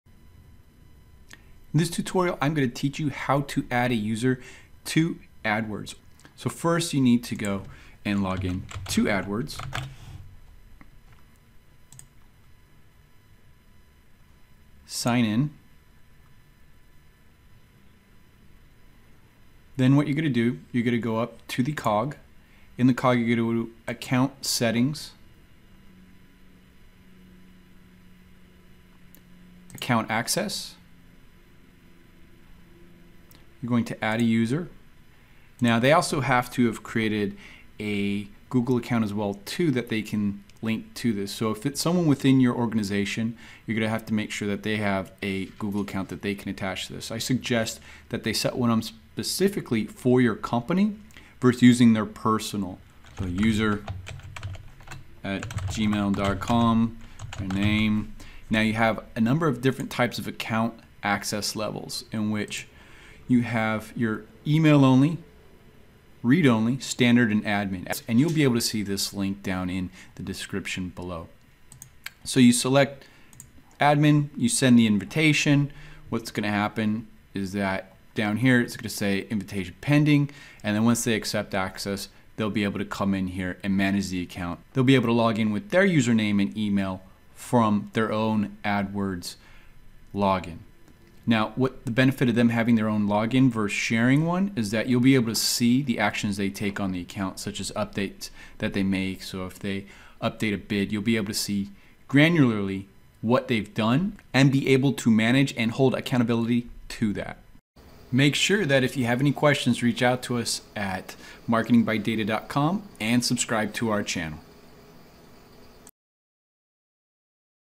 How To Add a User In Google Adwords and the Difference in Account Access
You need to be able to add a user in Adwords. This is different from having your marketing agency or adding someone who has an MCC (My Client Center) account.

Login to Adwords
Select the cog in the upper right corner
Select Account Settings
In the upper left select Account Access
Input the email and access level